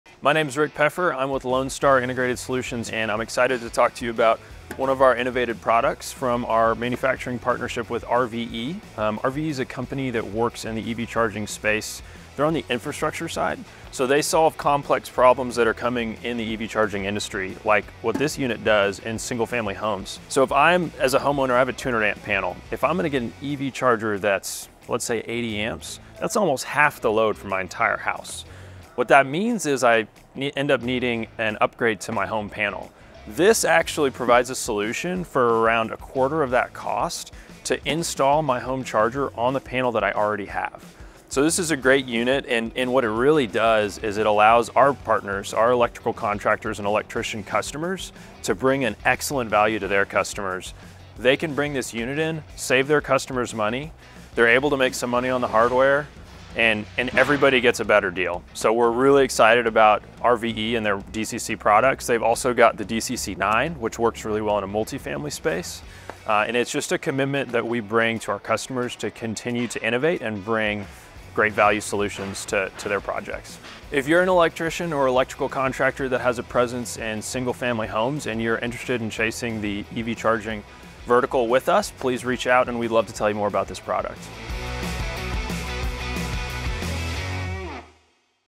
DCC-10 by RVE
Partner with Lonestar for your EV solutions!
Hear from Rick about the DCC-10 by RVE that allows a charger to be connected directly to an electrical panel which would otherwise not have sufficient capacity to allow the connection. Reach out to your local Lonestar to learn more about the different services and products our team provides for your next EVSE project.